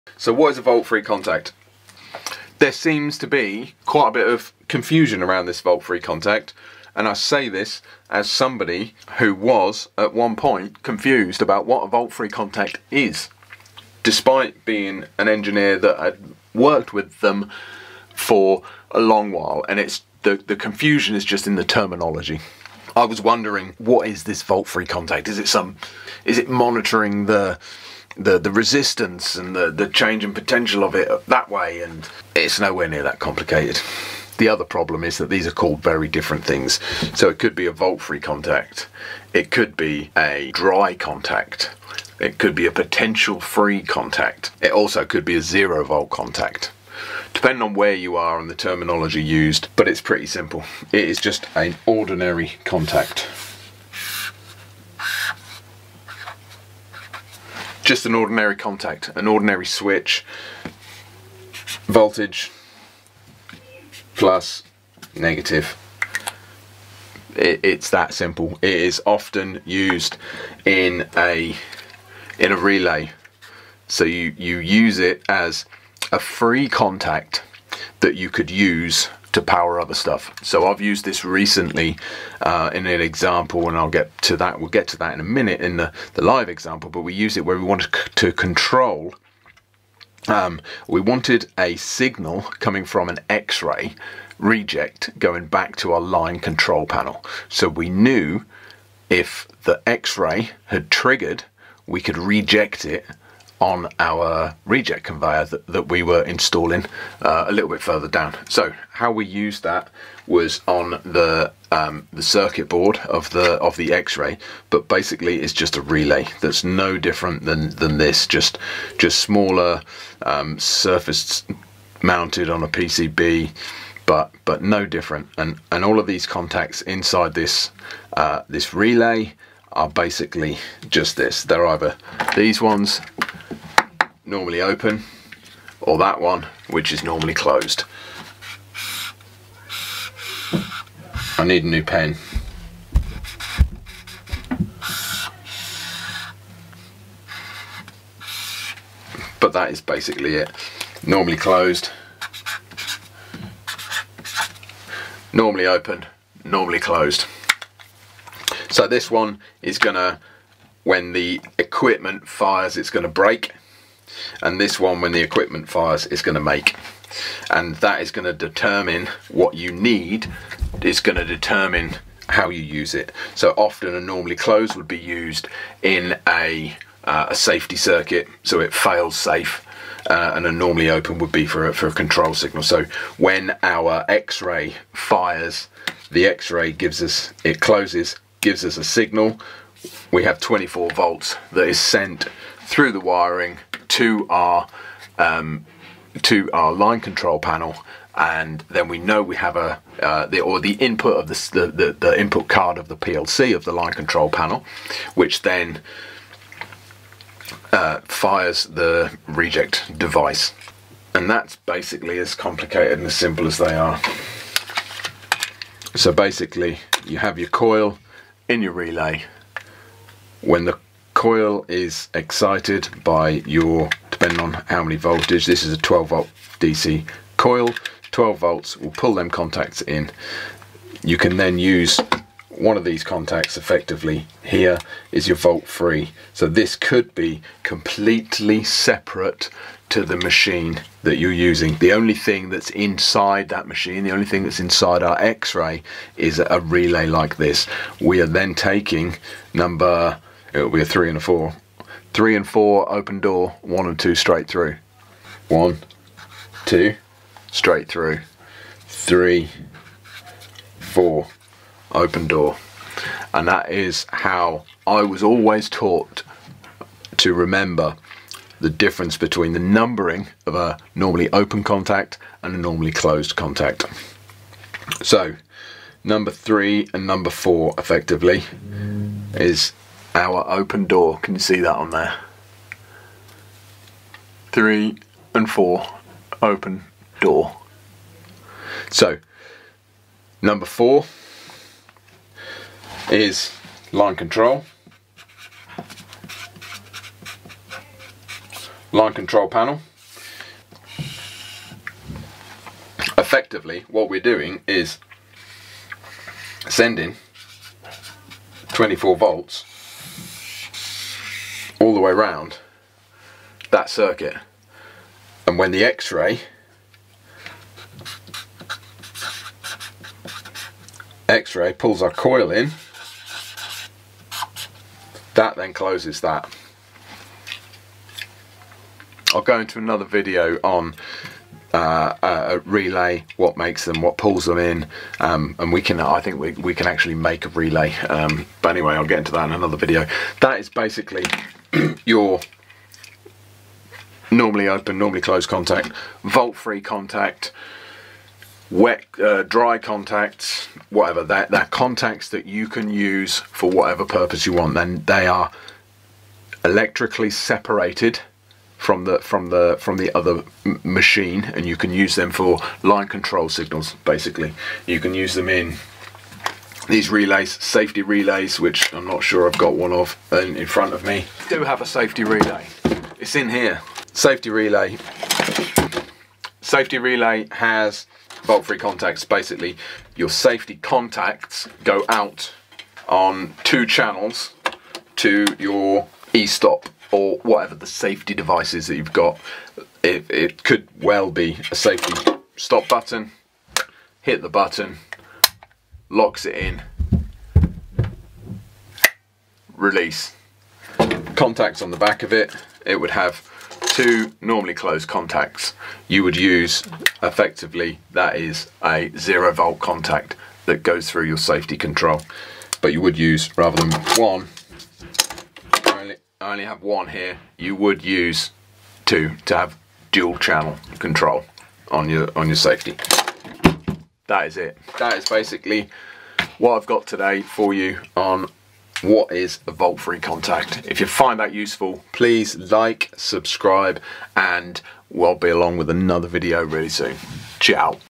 Zero Volt Contact explained
In this video I explain the zero volt contact, or you may of heard it called the volt free contact or a dry contact or a potential free contact.

Whatever you have heard it called, I explain it in this video and give a recent example of where we have used it in a factory control circuit.